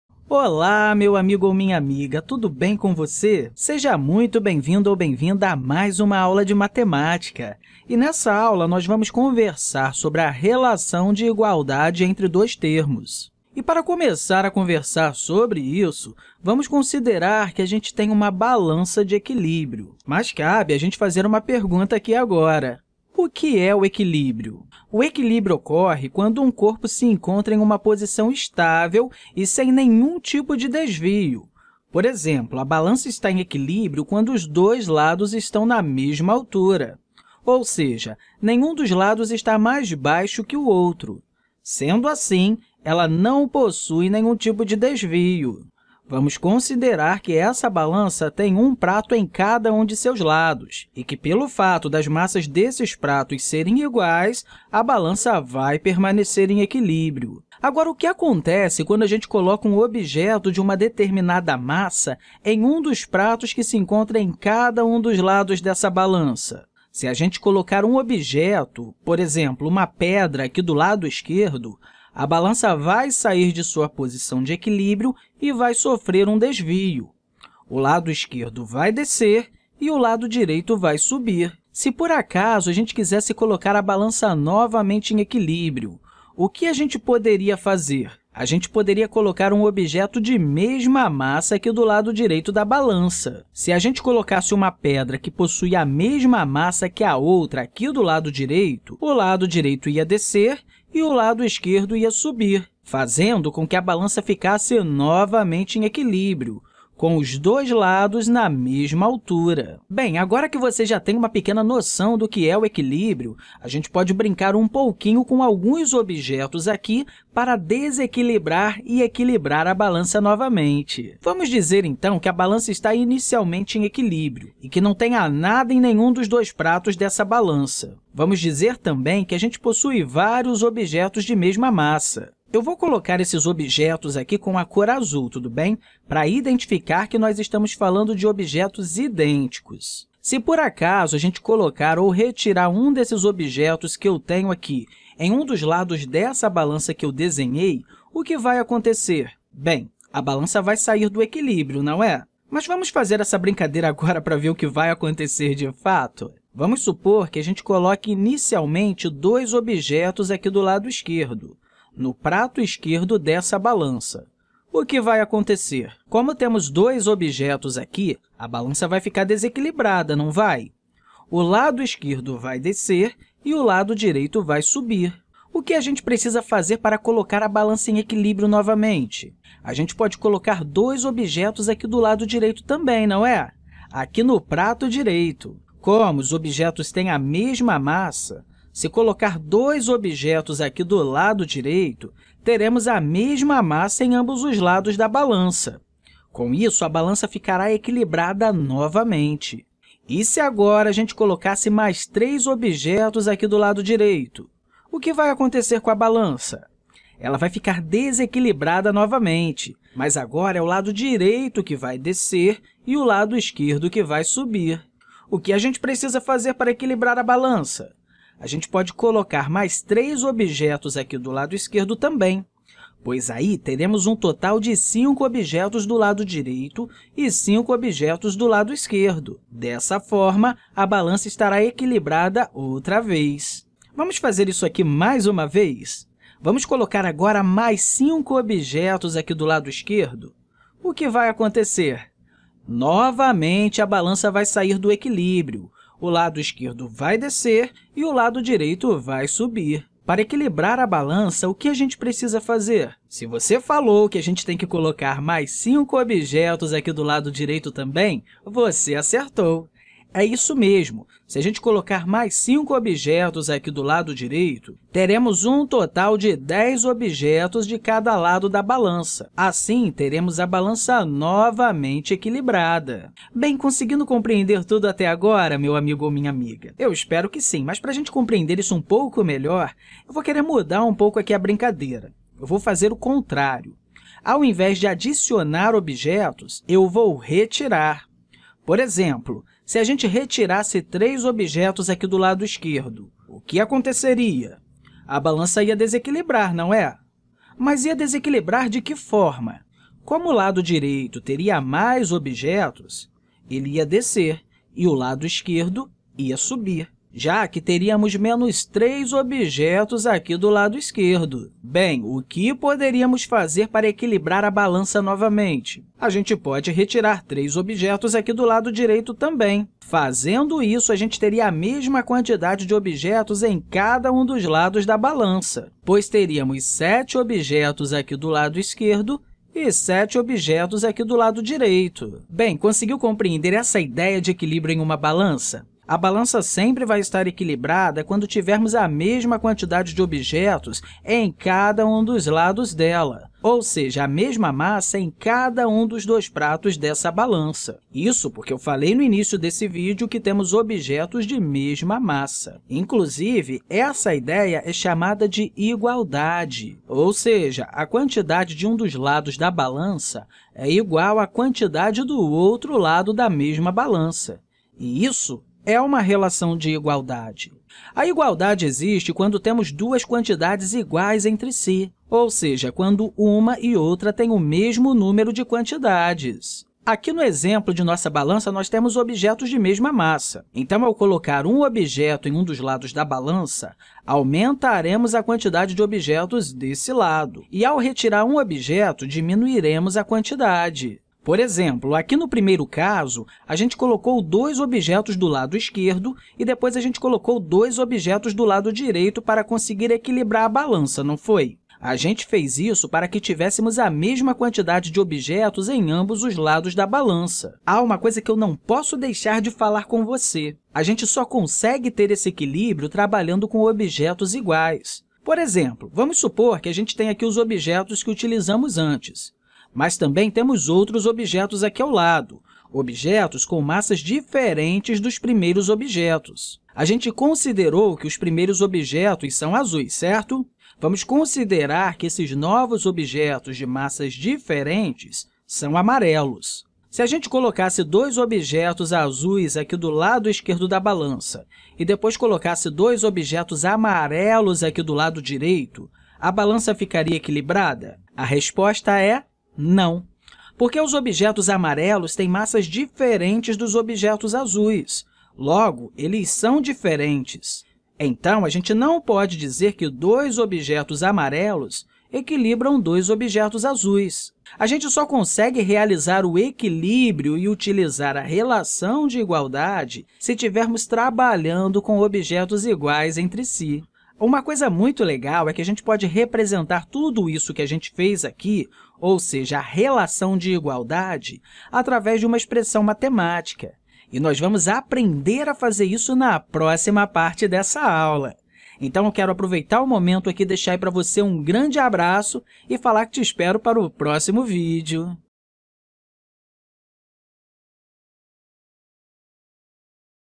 Relação de igualdade entre dois termos - balança | Parte I
Aprenda de forma lúdica o que é equilíbrio e como podemos mantê-lo no exemplo com balanças.